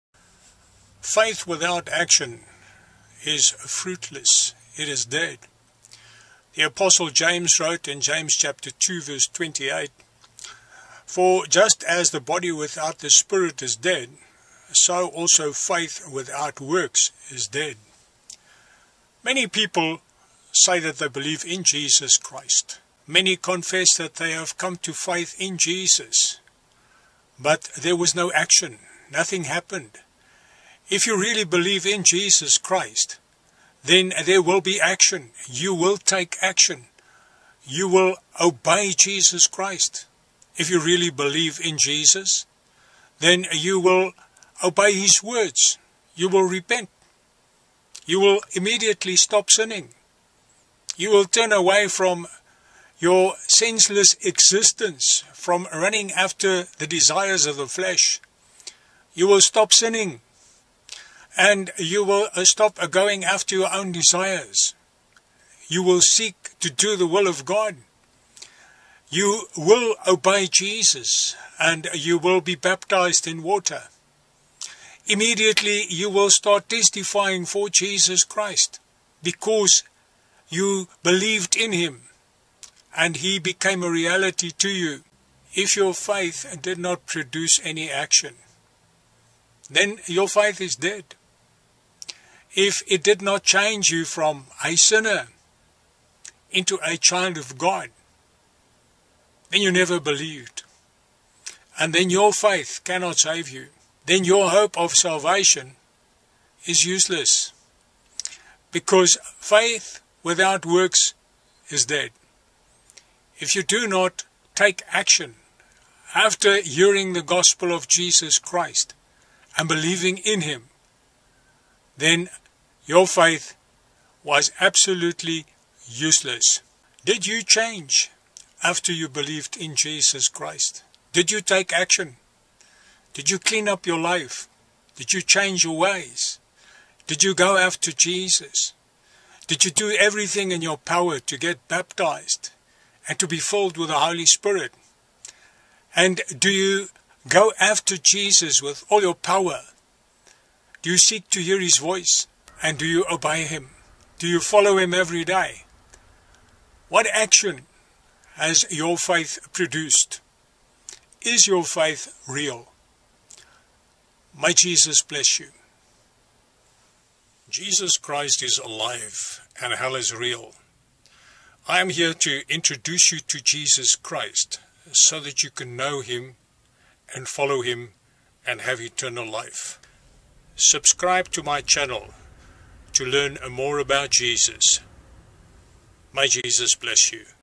Faith in Action
Faith without action is fruitless, it is dead. The Apostle James wrote in James chapter 2 verse 26:"For just as the body without the spirit is dead so also faith without works is dead." Many people say that they believe in Jesus Christ, many confess that they have come to faith in Jesus but there was no action. Nothing happened. If you really believe in Jesus Christ then there will be action. You will take action. You will obey Jesus Christ, if you really believe in Jesus then you will obey His words, you will repent. You will immediately stop sinning. You will turn away from your senseless existence, from running after the desires of the flesh. You will stop sinning and you will stop going after your own desires. You will seek to do the will of God. You will obey Jesus and you will be baptized in water, immediately you will start testifying for Jesus Christ, because you believed in Him and He became a reality to you.
If your faith did not produce any action, then your faith is dead. If it did not change you from a sinner into a child of God, then you never believed and then your faith cannot save you. Then your hope of
salvation is useless, because faith without works is dead. If you do not take action after hearing the gospel of Jesus Christ and believing in Him then your faith was absolutely useless.
Did you change after you believed in Jesus Christ? Did you take action? Did you clean up your life? Did you change your ways? Did you go after Jesus? Did you do everything in your power to get baptized and to be filled with the Holy Spirit, and do you go after Jesus with all your power? Do you seek to hear His voice and do you obey Him? Do you follow Him every day? What action has your faith produced? Is your faith real.